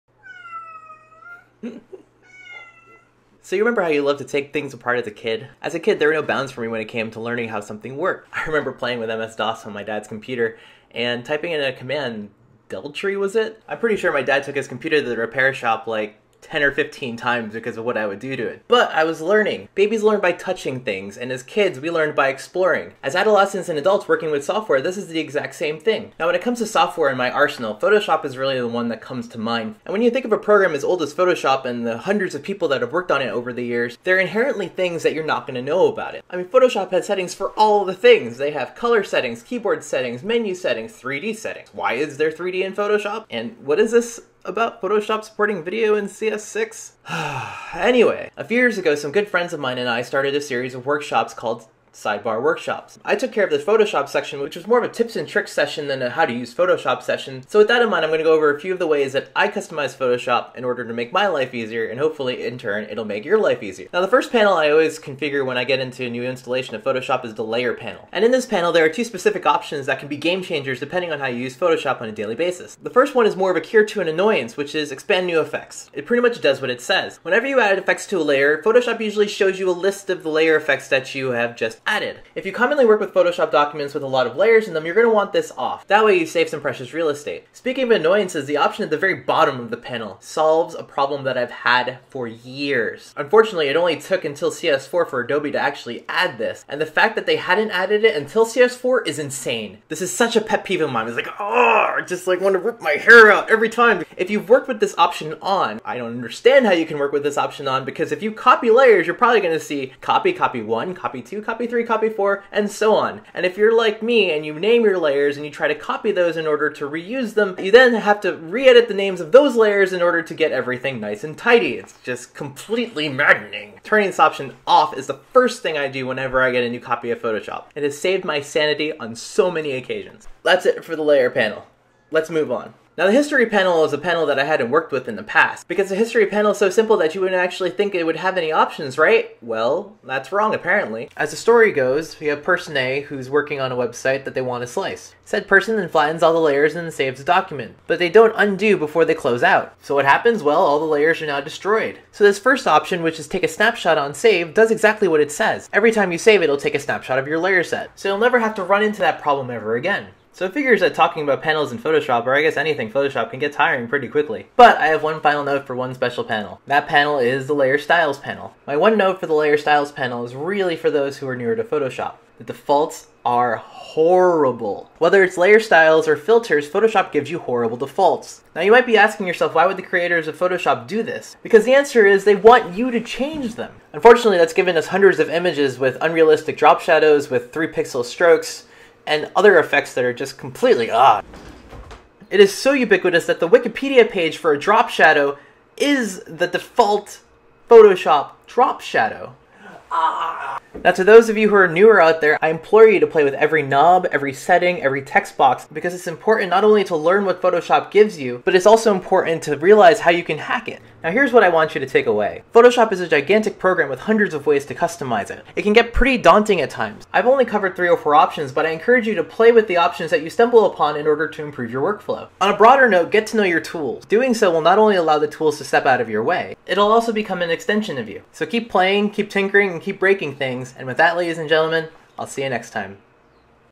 On Photoshop, Defaults and Panel Options
This is the short story of a designer's dealings with Photoshop and all of its... quirks. Said designer gives you a few quick tips on (hopefully) making your Photoshop experience better, implores you to play around with your settings and asks you to never use the defaults that Photoshop gives you.